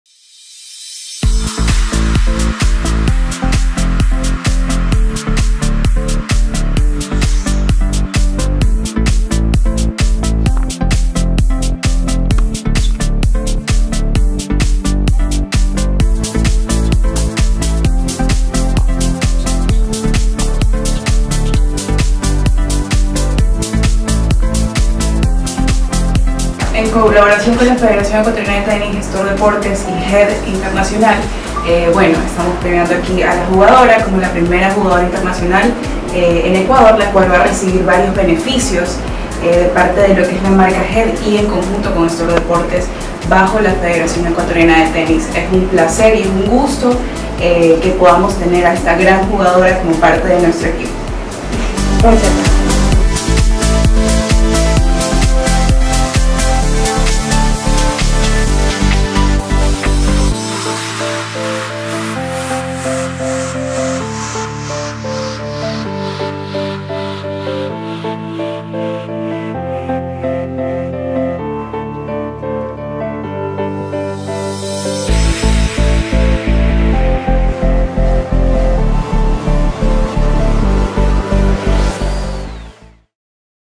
TENISTA VALENTINA VARGAS FIRMÓ CONVENIO DE AUSPICIO
Es miércoles la tenista Valentina Vargas firmó un convenio de auspicio con Store Deportes por dos años a través de la marca HEAD, también se alista para lo que será el clasificatorio al Australia Open Junior a jugarse este mes de noviembre en Brasil.